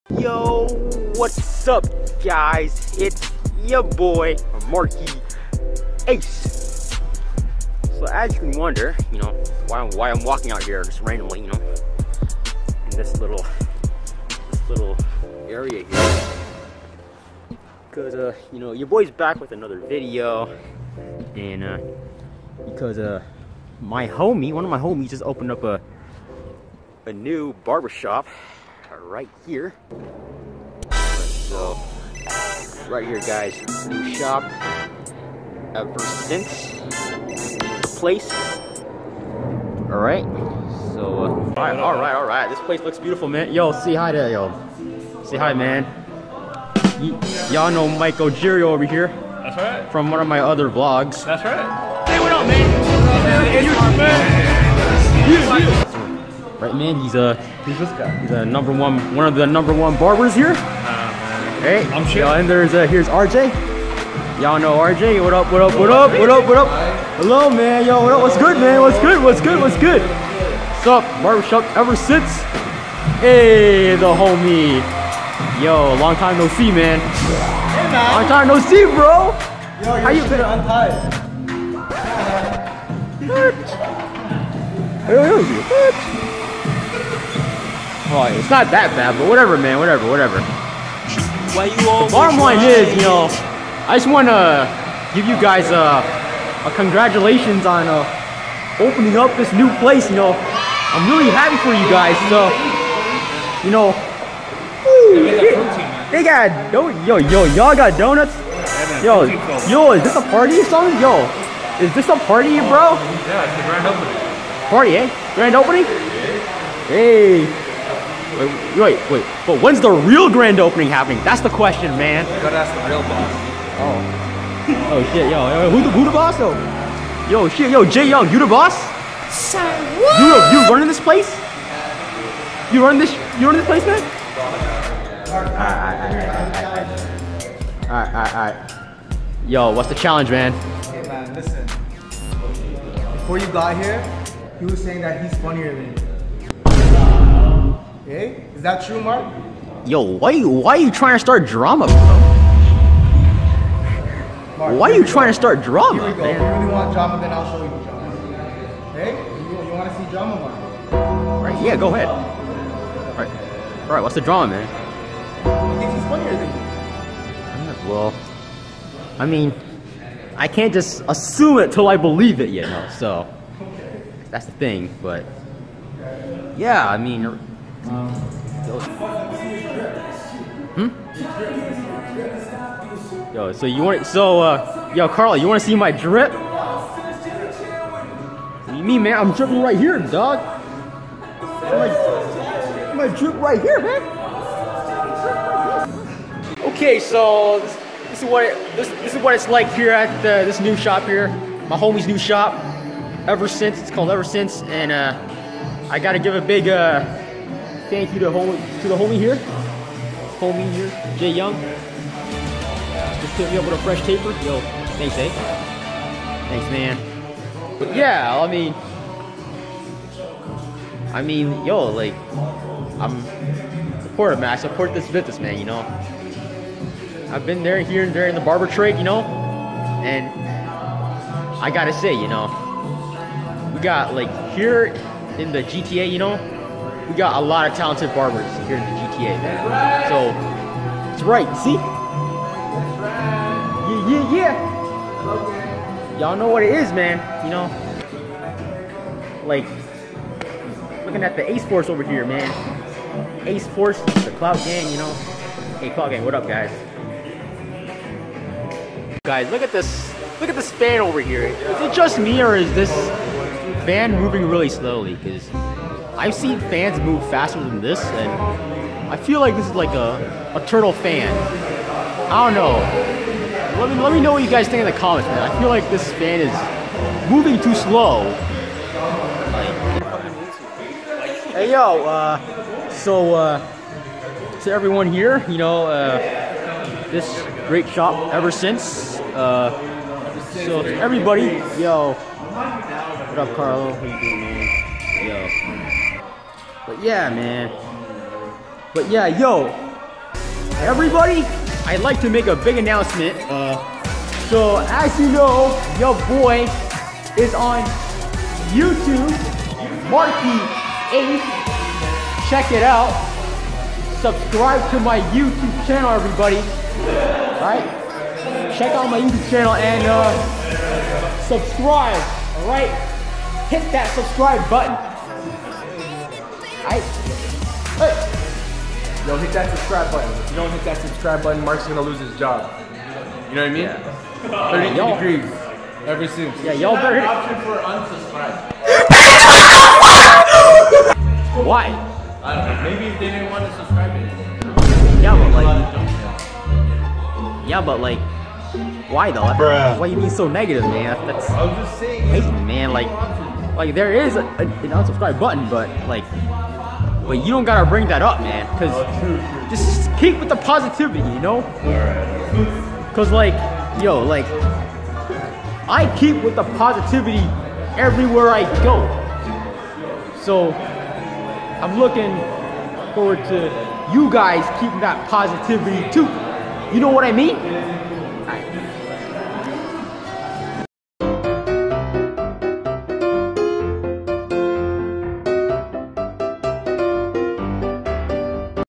Going to a new Barbershop!
Some of my homies opened up a new barbershop, had to check it out!